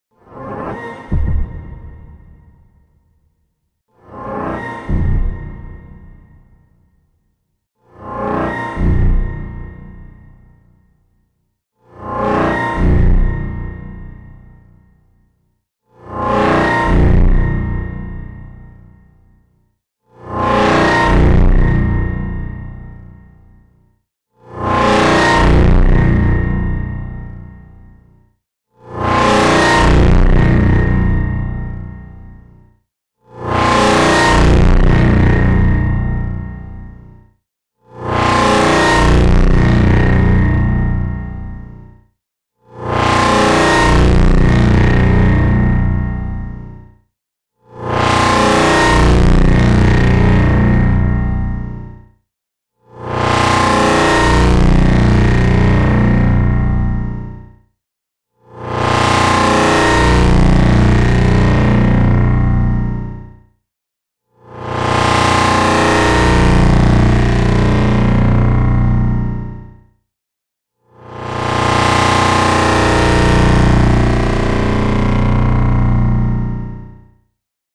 TGSMWI4701 Nissan Styled SNS logo but it's 1,000,000,000 times
Source made by @MilkyWay_174
decided to do 1 billion times on it

Loud warning !